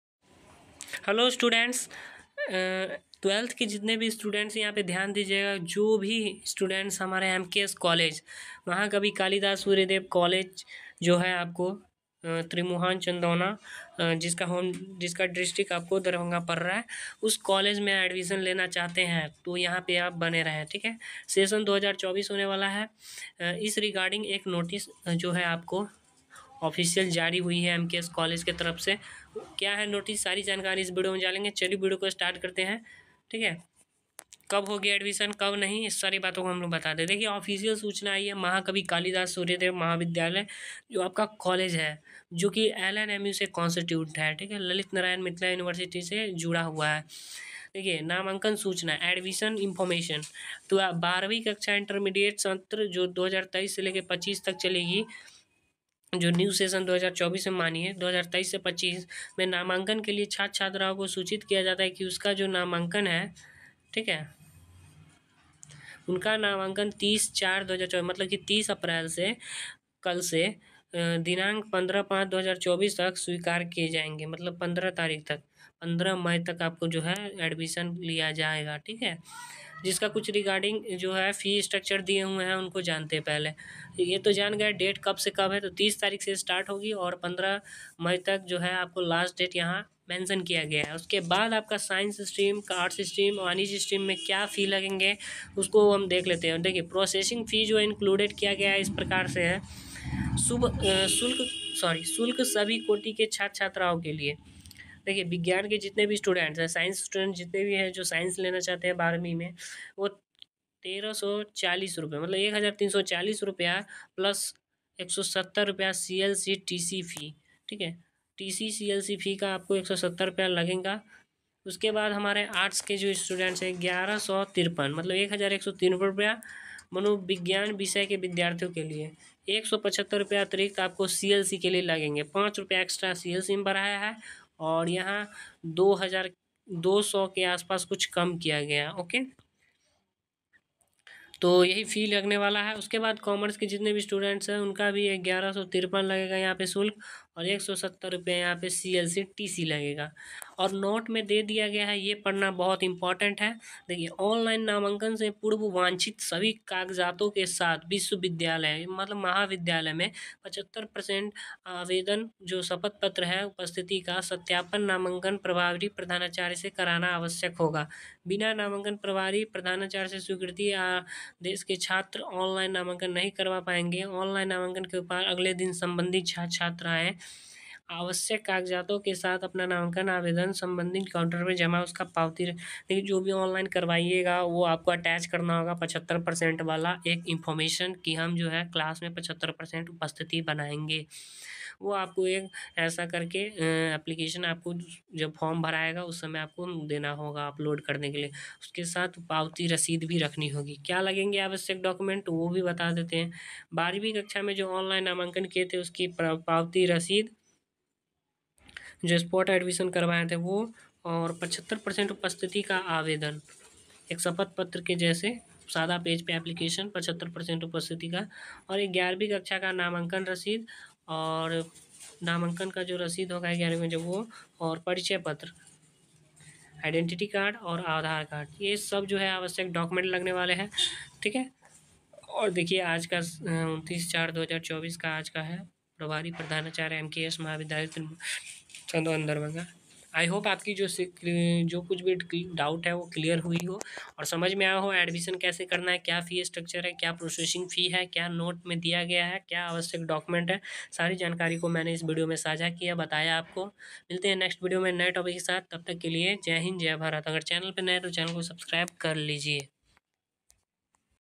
Mks college Chandauna 12th admission 2024 स्कूल कॉलेज में शुरू
Mks college chandauna darbhanga admission 12th Starts 🔥🔥🎓